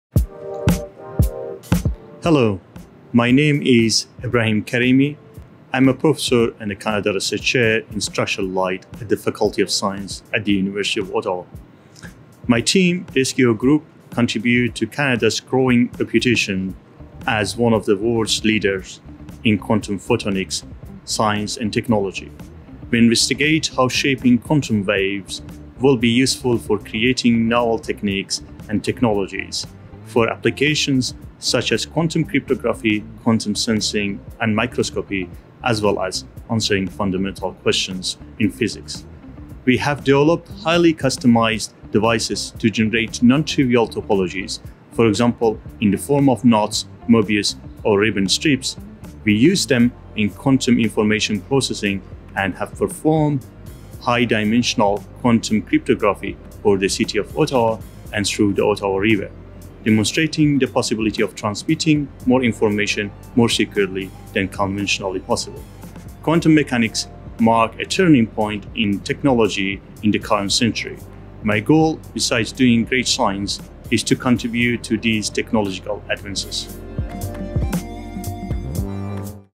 Ebrahim Karimi | Department of Physics | University of Ottawa
Ebrahim Karimi, Canada Research Chair at the University of Ottawa, is an international leader in the field of structured quantum waves. He explores how photons, the tiniest particles of light, and electrons can be shaped to carry specific quantum properties. These quantum features were employed in quantum communication demonstrations over the city of Ottawa and in the Ottawa river, ground-breaking techniques in microscopy, and answering fundamental questions in physics.

Ebrahim Karimi, titulaire de la Chaire de recherche du Canada à l’Université d’Ottawa, est un leader international dans le domaine des ondes quantiques structurées. Il explore la façon dont les photons, les plus petites particules de lumière, et les électrons peuvent être manipulés pour obtenir des propriétés quantiques particulières. Ces propriétés ont été employées dans des schémas de communication quantique à Ottawa, dans des techniques révolutionnaires de microscopie, ainsi que pour répondre à des questions fondamentales en physique.